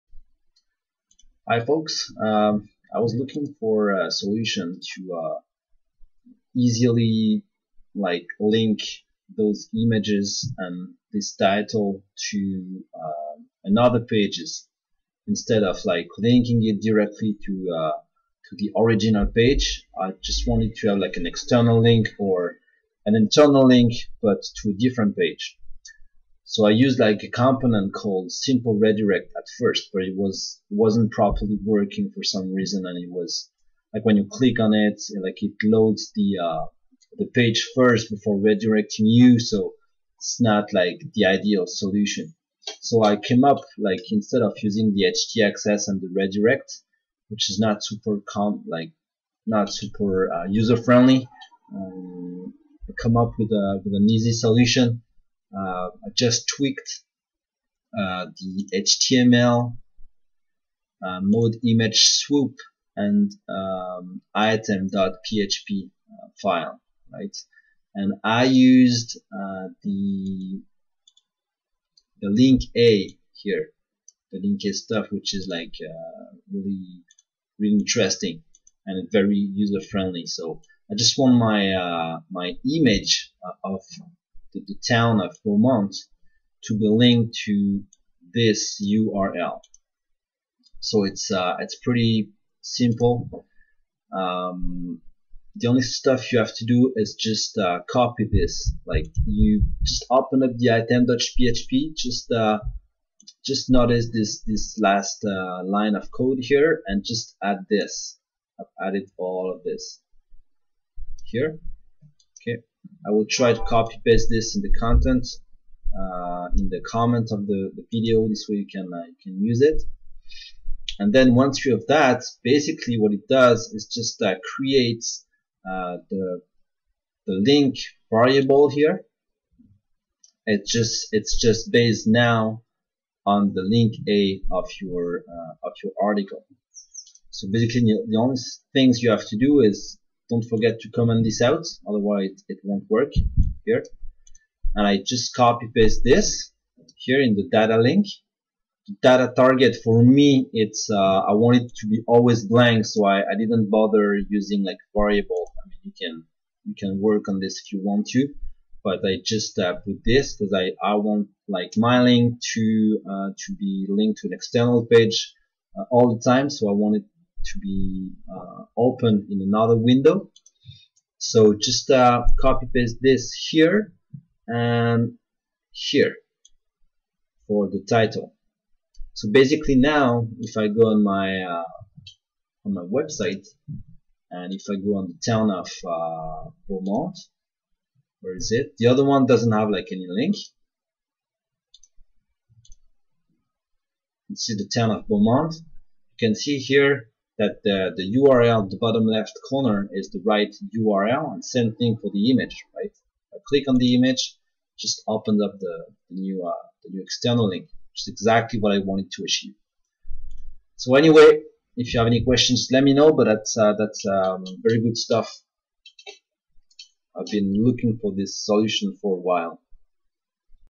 Custom link on slideshow images Joomla (example on Image Swoop modules)
Use this tutorial to tweak your _item.php to get the Joomla link A of your article used for the link of your slideshow image. You can find a portion of the code used in the video on this page: